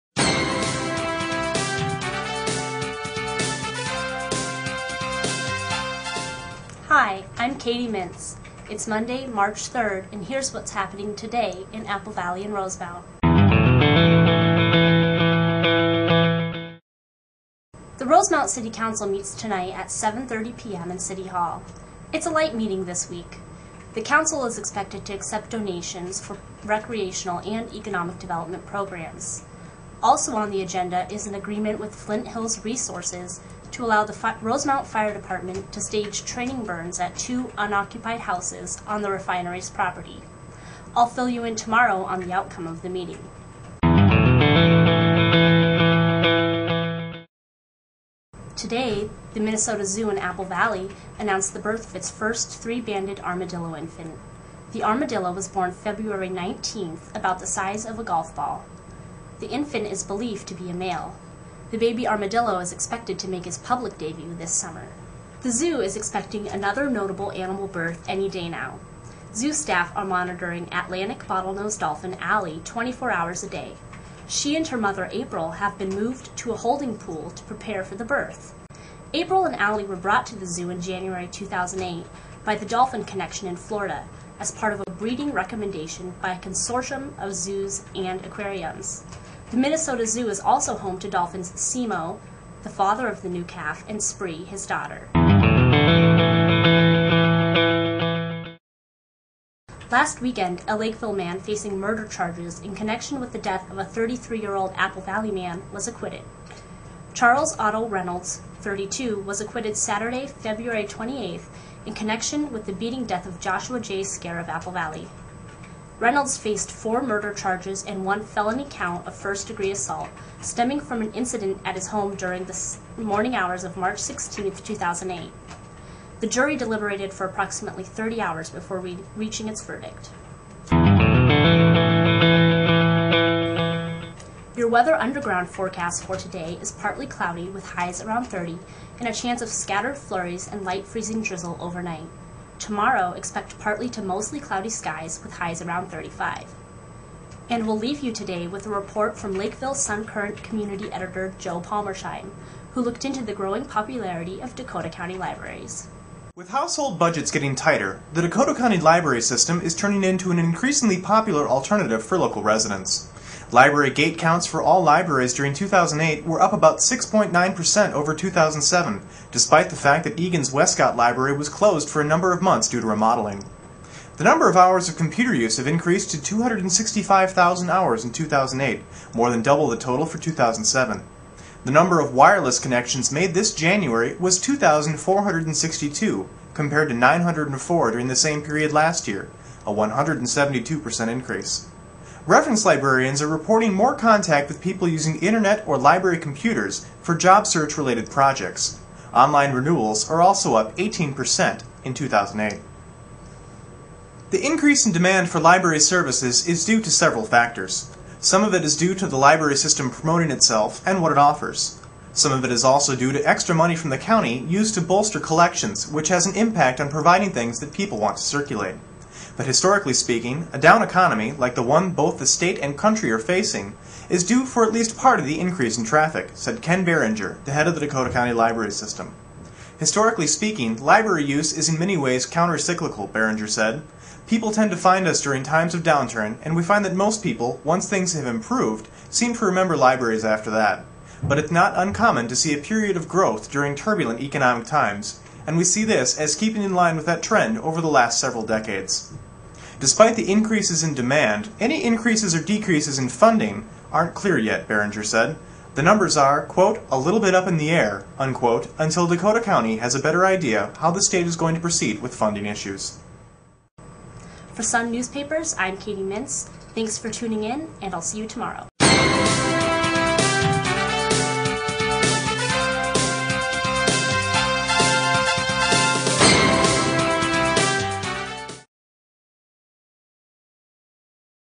Apple Valley-Rosemount Today Tuesday, March 3, webcast
Zoo announces armadillo birth, dolphin on the way

Today the Minnesota Zoo in Apple Valley announced the birth of its first three-banded armadillo infant. The armadillo was born Feb. 19 about the size of a golf ball. The infant is believed to be a male. The baby armadillo is expected to make his public debut this summer.

The Zoo is expecting another notable animal birth any day now. Zoo staff are monitoring Atlantic bottlenose dolphin Allie 24 hours a day. She and her mother April have been moved to a holding pool to prepare for the birth.

April and Allie were brought to the Zoo in January 2008 by the Dolphin Connection in Florida as part of a breeding recommendation by a consortium of zoos and aquariums. The Minnesota Zoo is also home to dolphins Semo, the father of the new calf, and Spree, his daughter.